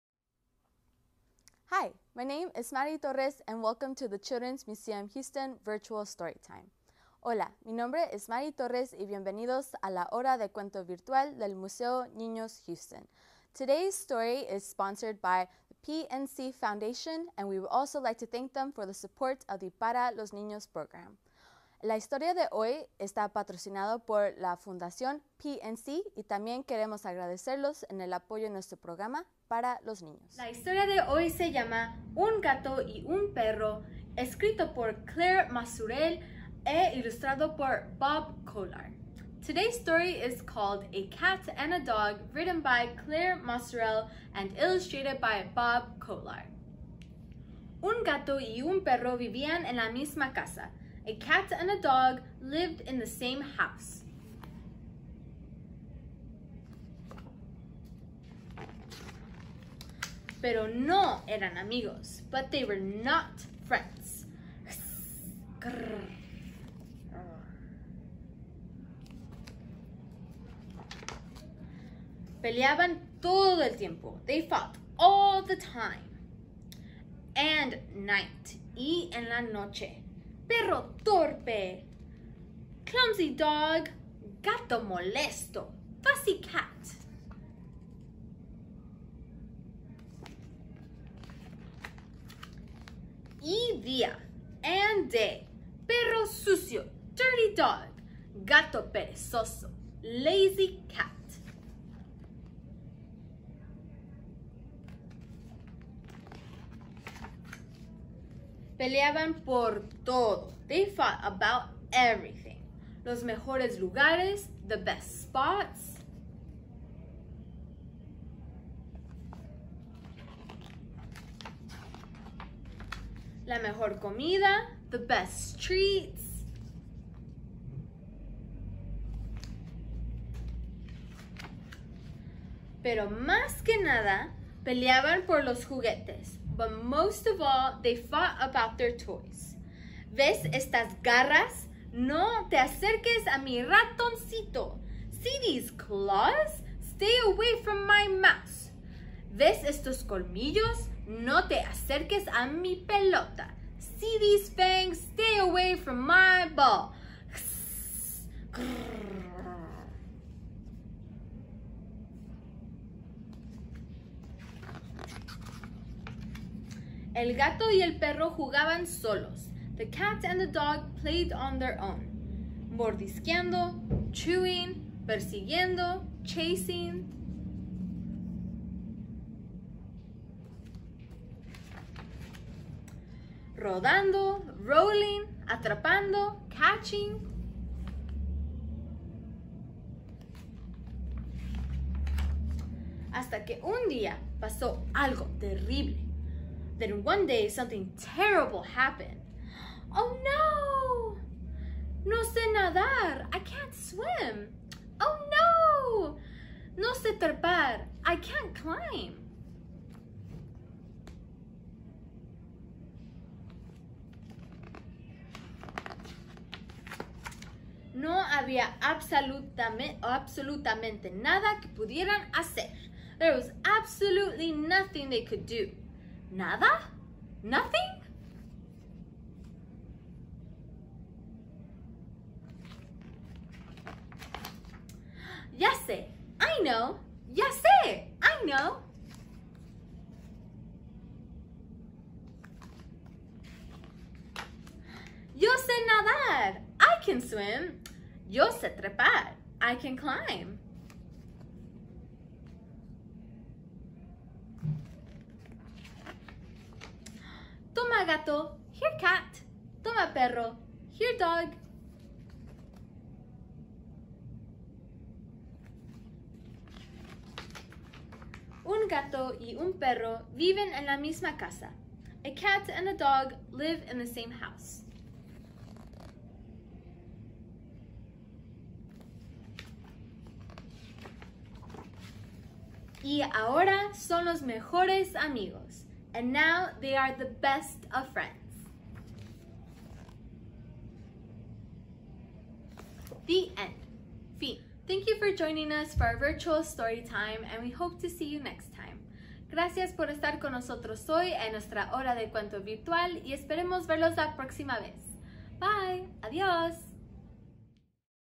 Un Gato y Un Perro - Bilingual Storytime
What's the truth about cats and dogs?  Follow along as Children's Museum Houston educator Maddie explains the cat-dog relationship in "A Cat and a Dog" (Un gato y un perro) by Claire Masurel in English and Spanish.

Storytime is part of our "All-Time Access" program to connect you with our educators wherever you are - at school, at home, at play!